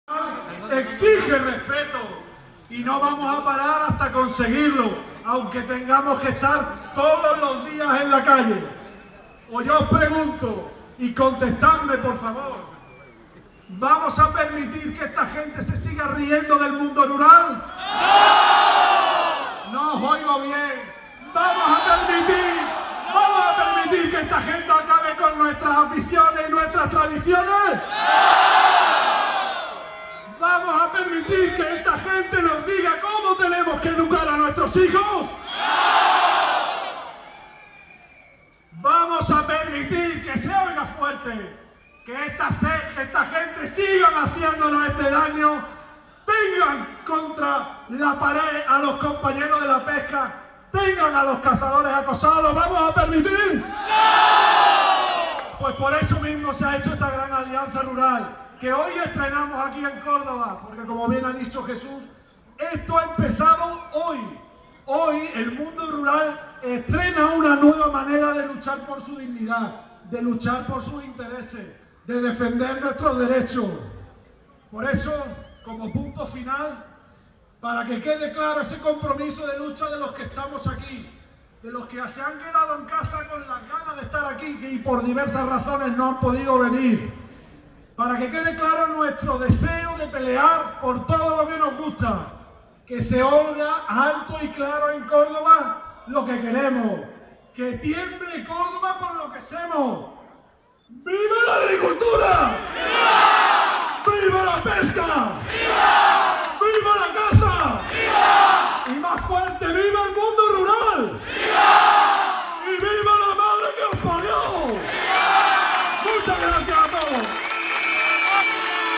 José Mª. Mancheño en la manifestación en defensa del mundo rural en Córdoba 30/09/2017 (y II)
Discurso de José Mª. Mancheño en la manifestación en defensa del mundo rural en Córdoba 30/09/2017.
Segunda parte.